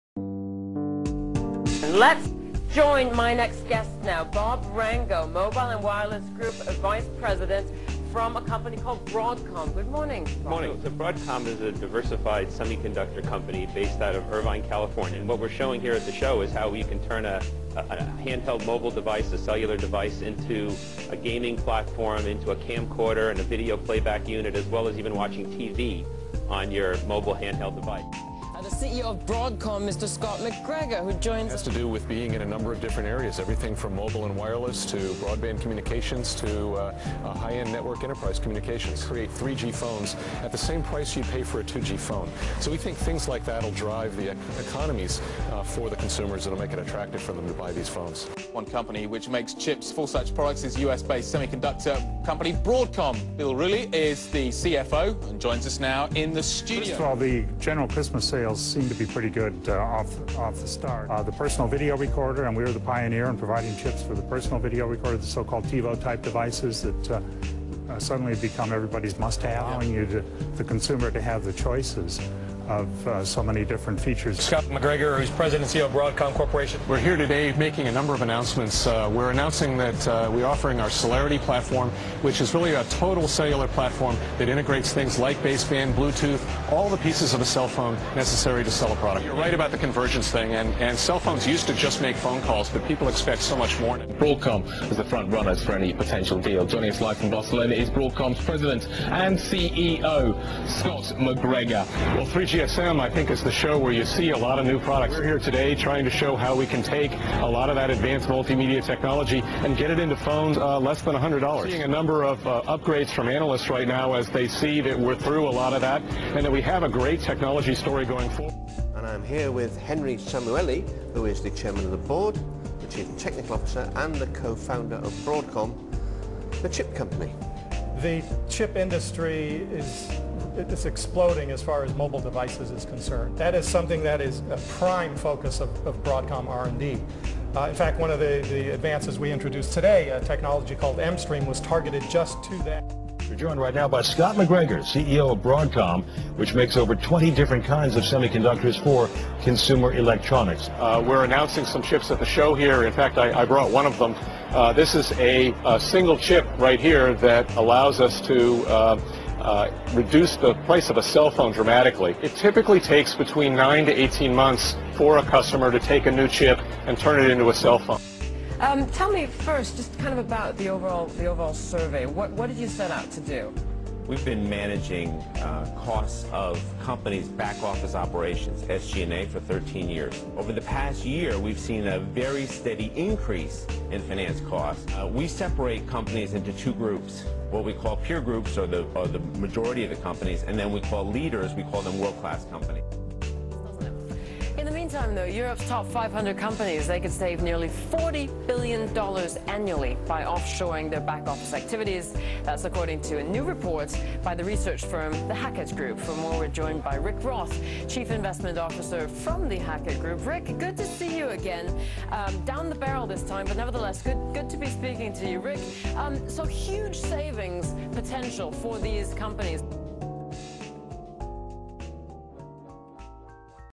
Sherpa Media: Credentials Video
Sherpa Media is a London, UK-based PR consultancy.  Here's some examples of high visibility media coverage we've generated for clients like Broadcom and The Hackett Group.